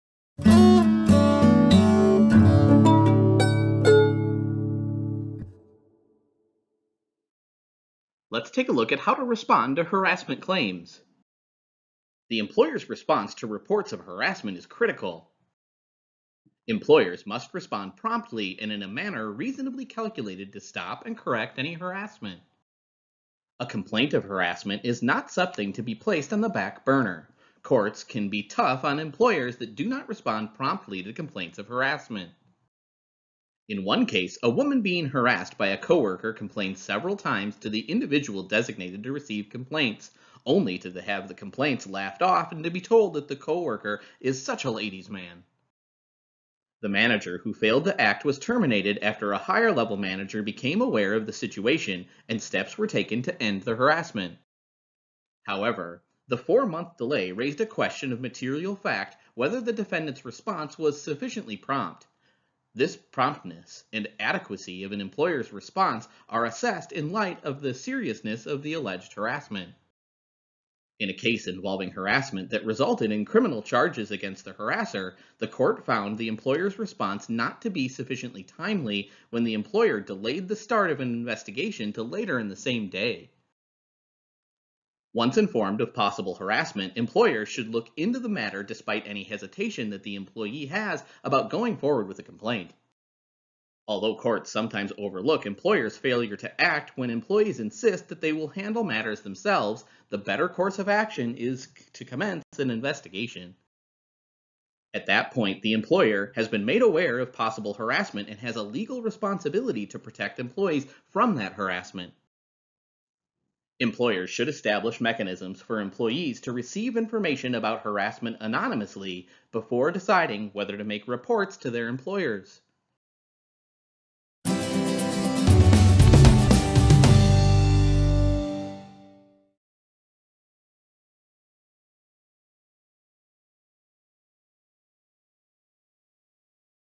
Responding to Harassment Claims
The employer’s response to reports of harassment is critical. Employers must respond promptly and in a manner reasonably calculated to stop and correct any harassment. A complaint of harassment is not something to be placed on the “back burner.” Courts can be tough on employers that do not respond promptly to complaints of harassment.

In one case, a woman being harassed by a coworker complained several times to the individual designated to receive complaints, only to have the complaints laughed off and to be told that the coworker is such a ladies man. The manager who failed to act was terminated after a higher-level manager became aware of the situation and steps were then taken to end the harassment. However, the four-month delay in acting raised “a question of material fact whether defendant’s response was sufficiently prompt.” The promptness and adequacy of an employer’s response are assessed in light of the seriousness of the alleged harassment.

In a case involving harassment that resulted in criminal charges against the harasser, the court found the employer’s response not to be sufficiently timely when the employer delayed the start of its investigation to later the same day. Once informed of possible harassment, employers should look into the matter despite any hesitation that the employee has about going forward with a complaint.  Although courts sometimes, as in the Hardage case, overlook employers’ failure to act when employees insist that they will handle the matter themselves, the better course of action is to commence an investigation.

At that point, the employer has been made aware of possible harassment and has a legal responsibility to protect employees from harassment. Employers should establish mechanisms for employees to receive information about harassment anonymously before deciding whether to make reports to their employers.